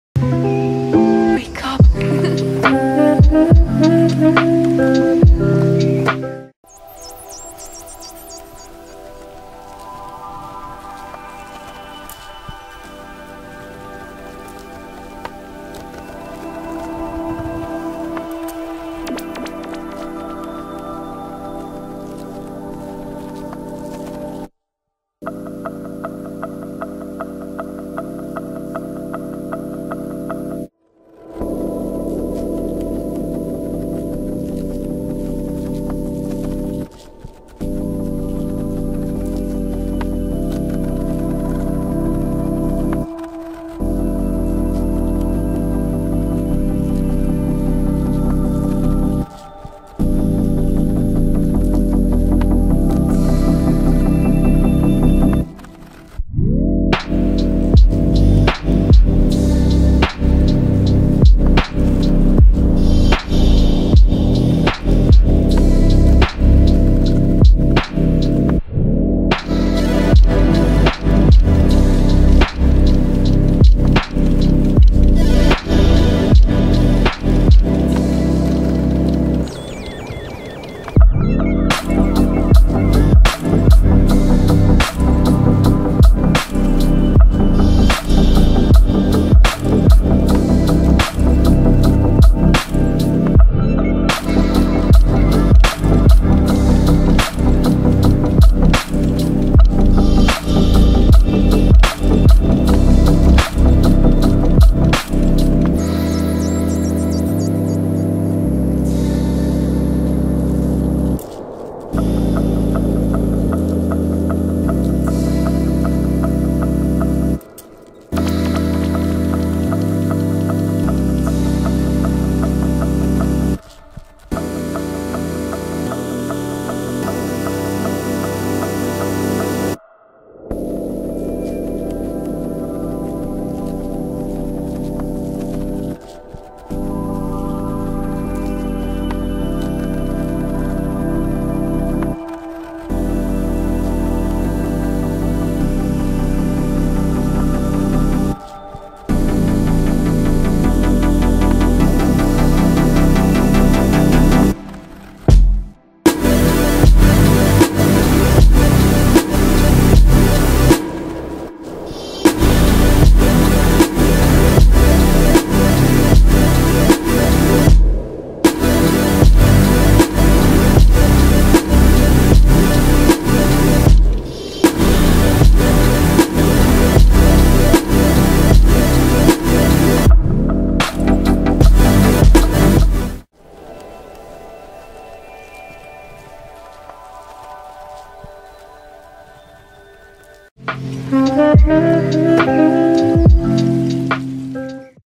ROBLOX Speed Edit: Summer Vibes
Holaaa, Its been a while since I've uploaded a speed edit, I really missed it. Also sorry for not adding the Blender part of this edit, I think I didn't record it thinking I did ._. lol
ANYWAYS hope you all are doing great! cyaaa
Made a group C:
ROBLOX: pamxla
Instragram: nxthing_0

I got this song from Aviencloud

-pam c;